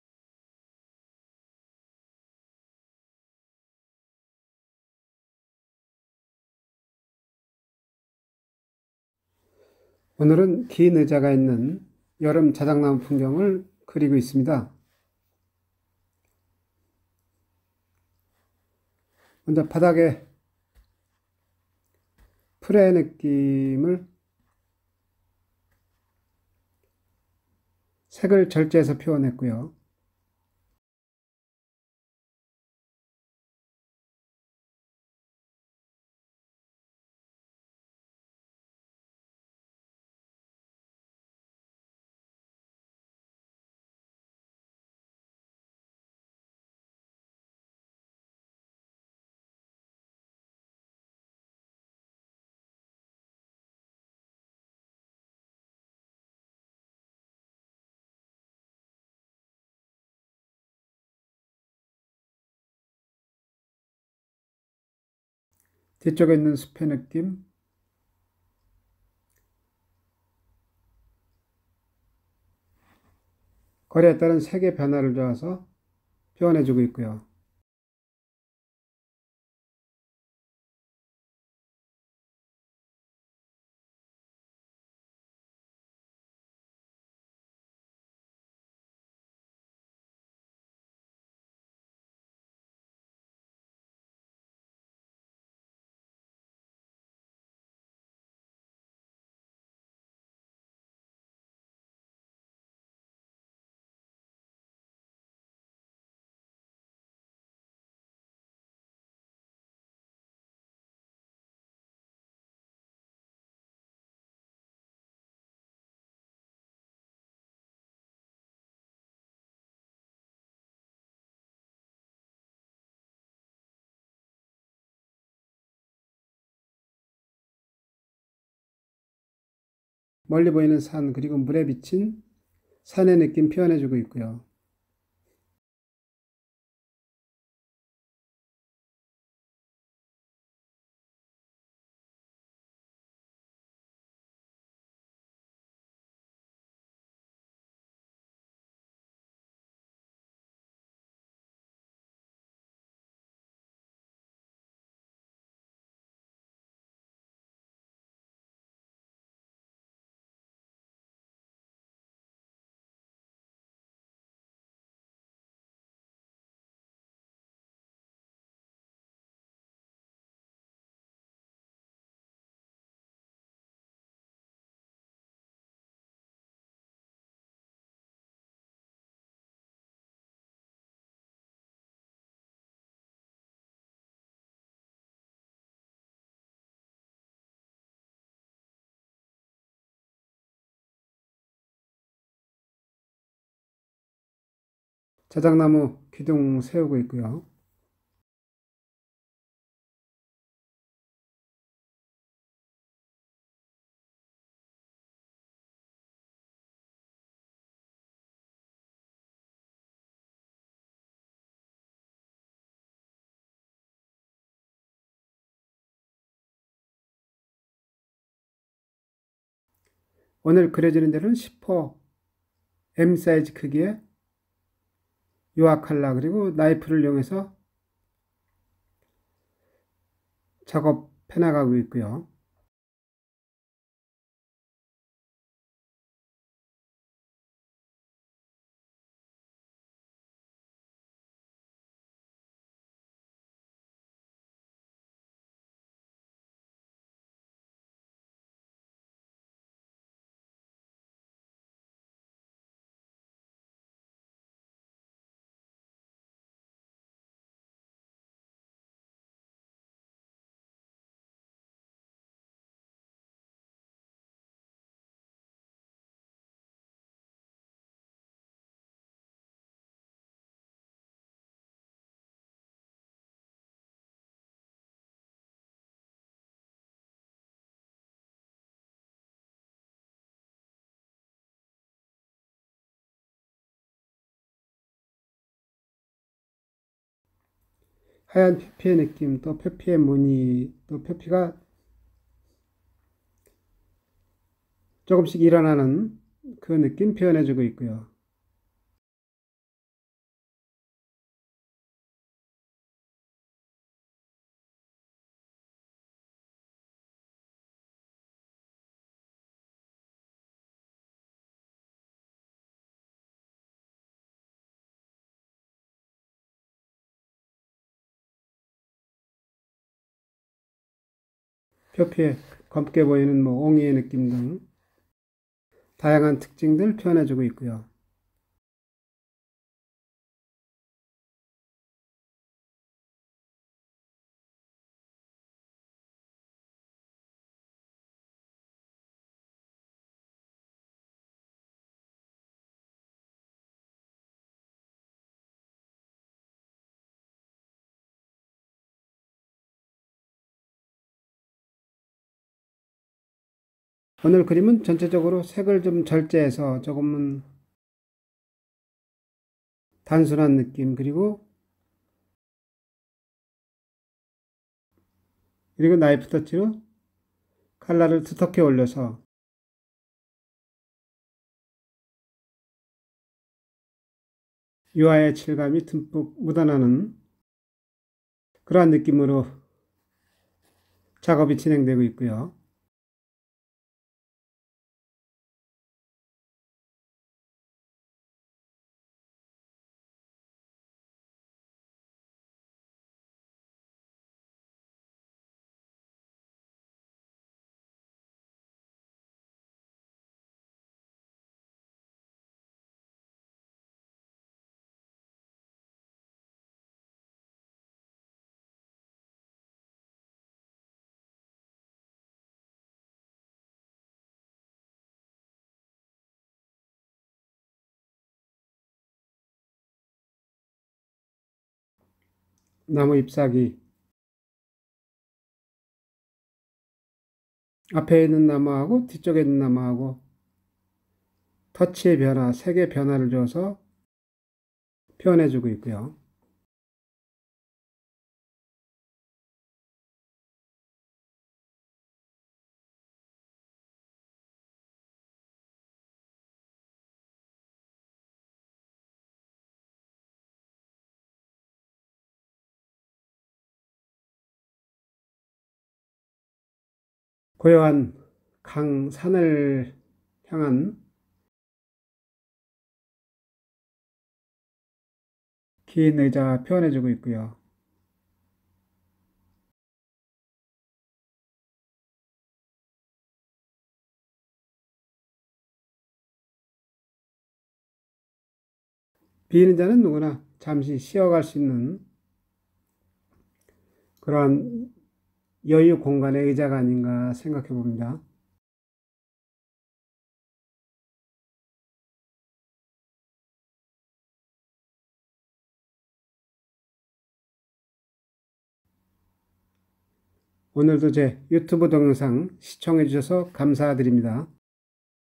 신재흥 20- 195/ 자작나무 숲(birch forest) 나이프로 그리기 / oil painting
여름 자작나무 풍경을 나이프로 그리기 시작해서 완성의 과정/ 유화칼라